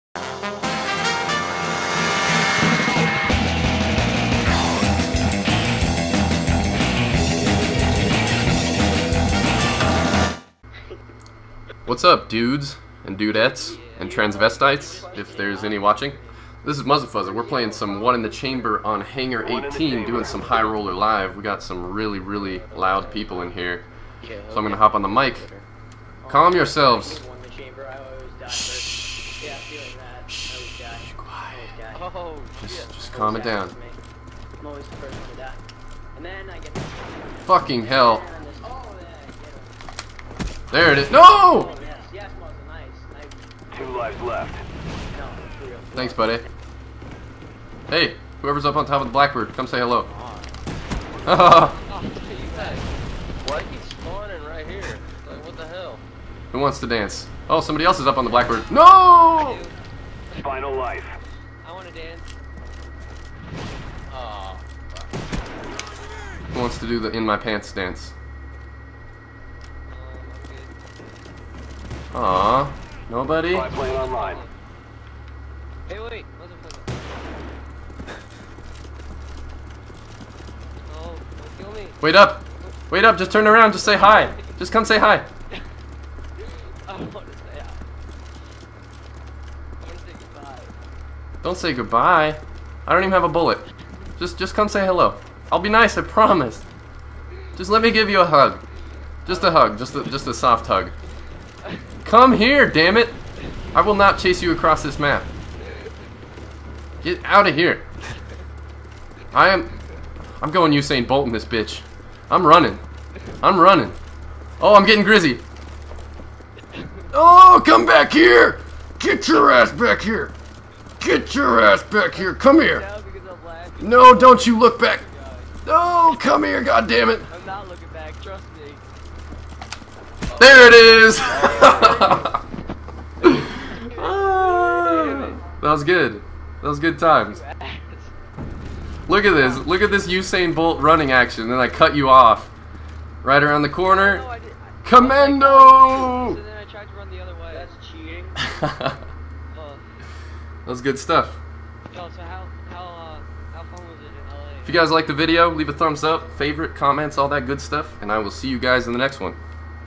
High Roller Live - Hangar 18 One in the Chamber
There's nothing like a high speed chase to end the game :)
If you support what I do and want to help out, download my toolbar.
It's got all my links to everything that I ever do, and more: